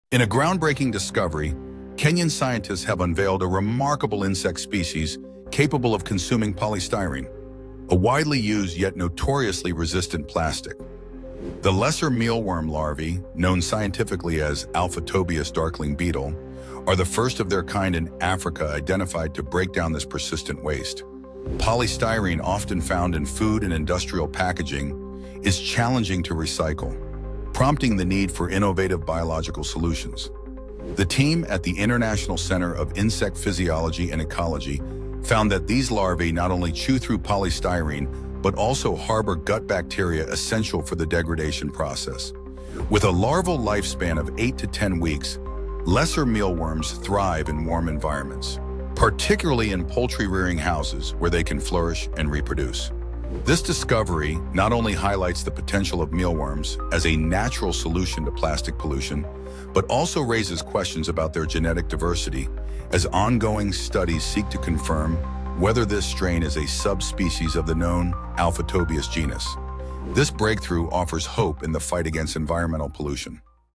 Nature's Plastic Eaters : Mealworms in Action
In a groundbreaking discovery, Kenyan scientists have unveiled a remarkable insect species capable of consuming polystyrene, a widely used yet notoriously resistant plastic. The lesser mealworm larvae, known scientifically as Alphitobius darkling beetle, are the first of their kind in Africa identified to break down this persistent waste. Polystyrene, often found in food and industrial packaging, is challenging to recycle, prompting the need for innovative biological solutions. The team at the International Centre of Insect Physiology and Ecology found that these larvae not only chew through polystyrene but also harbor gut bacteria essential for the degradation process.